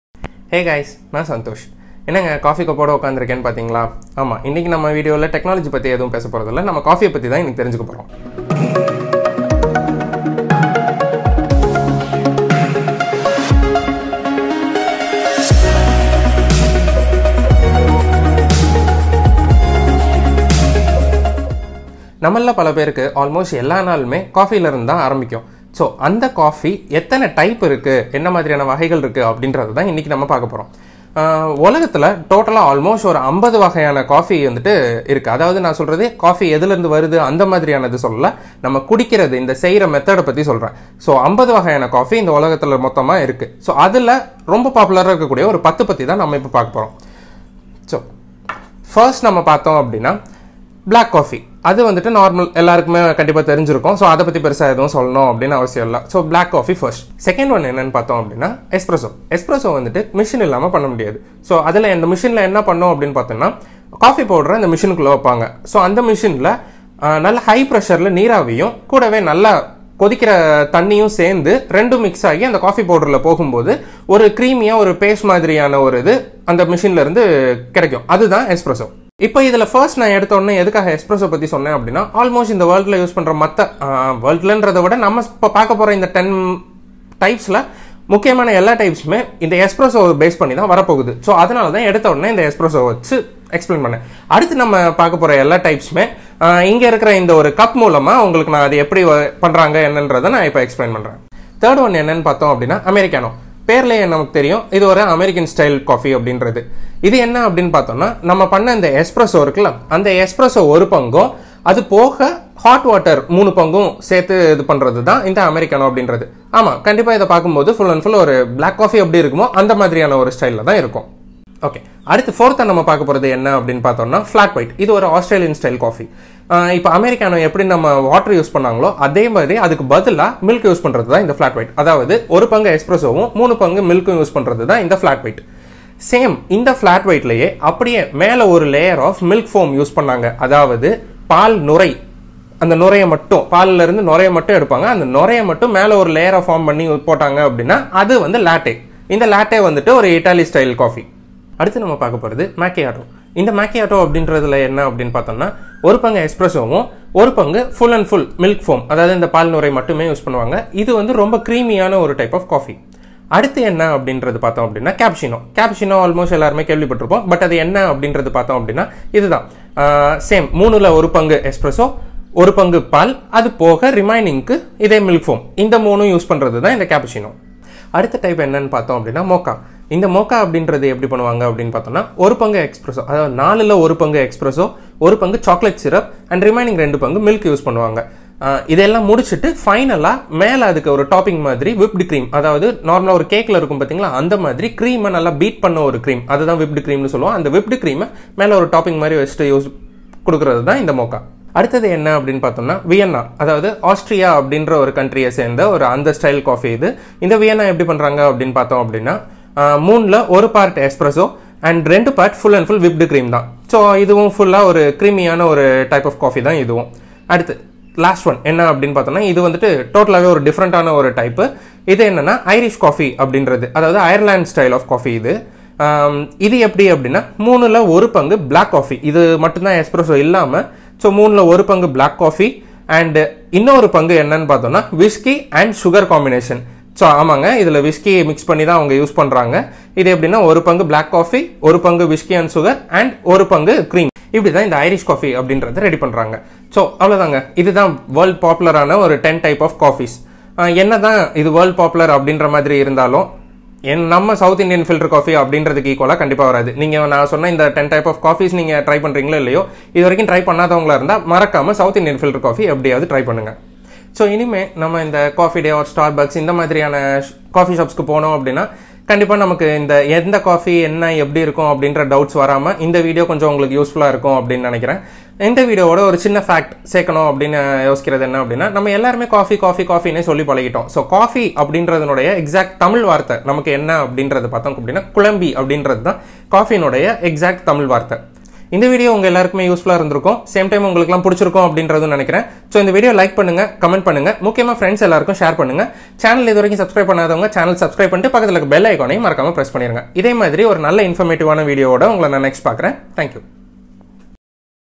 Varieties of Coffee | Let's Learn (Ep - 3) | Tamil | Santhosh Babu
In this video, we discuss about the different varieties of coffee available around the world.